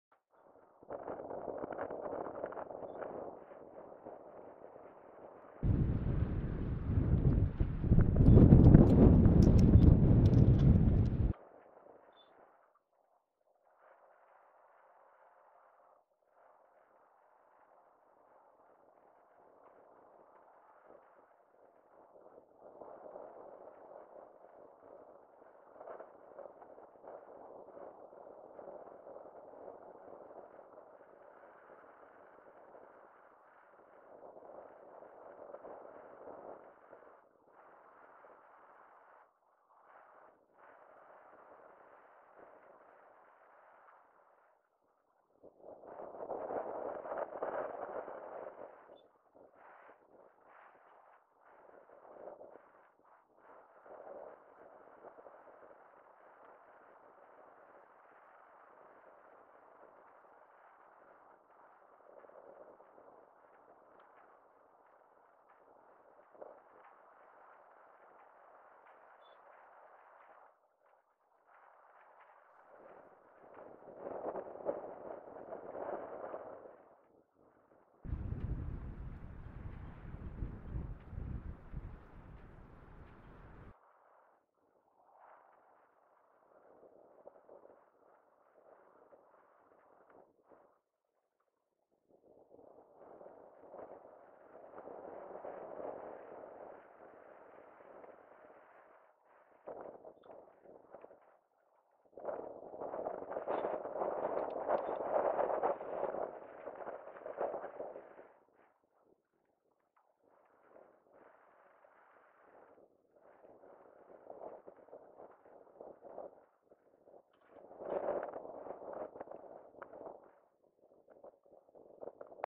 2 Minutes of Nature : 052917
2 Minutes of Nature - The phenomena of the physical world

Wind and flowers
Shelby Michigan

Wind and flowers at a rest area in Shelby Michigan.